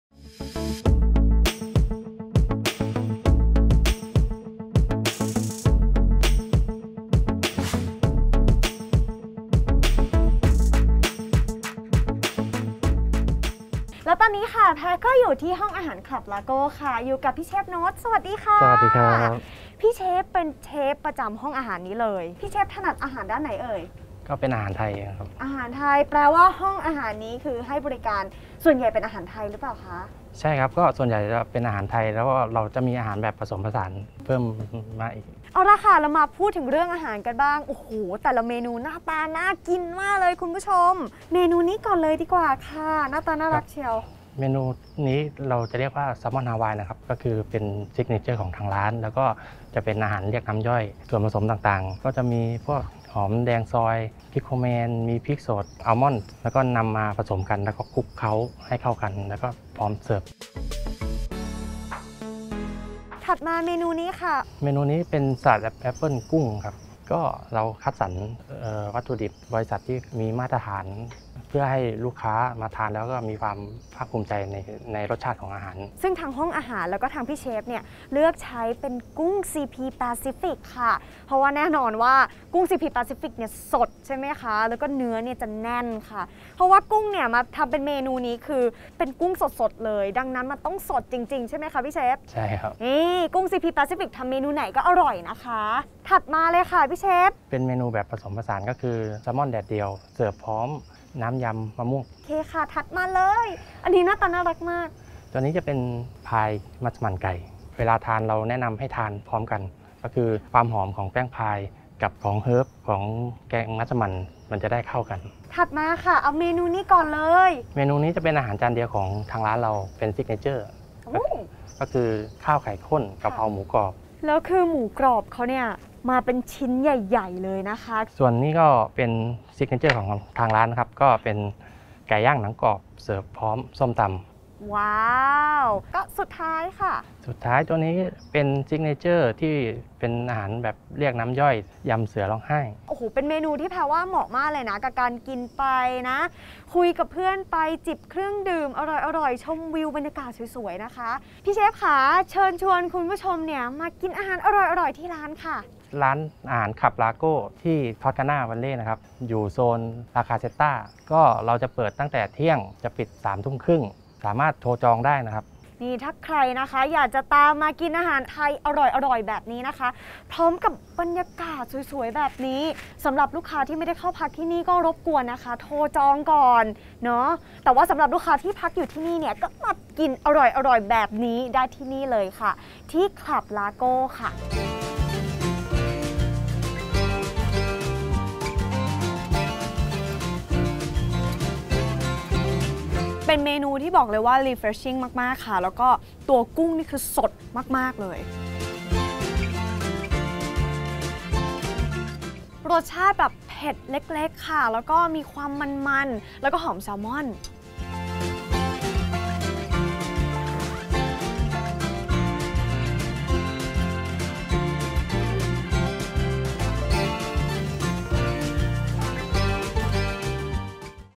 Club Lago by Toscana Valley ห้องอาหารกลางหุบเขา
ห้องอาหาร Club Lago โรงแรม Toscana Valley Khao Yai บรรยากาศเหมือนอยู่ในอิตาลี โดยเน้นการนำเสนอเมนู คอมฟอร์ดฟู้ด ง่ายๆ เพราะเชื่อว่าลูกค้าส่วนใหญ่ที่แวะมาตั้งใจมาพักผ่อน เมนูอาหารที่นี่จึงเป็นเมนูที่หลายคนคุ้นเคย
ร้านเปิดทุกวัน เวลา 7:00–22:00 น.

อย่าลืมกดติด ⭐️ เพจของเราด้วยนะคะ

ช่องทางอื่นๆ ที่สามารถติดตามความเคลื่อนไหว ที่ไหนมีที่เที่ยวใหม่ๆ ของกินเด็ดๆ ได้ที่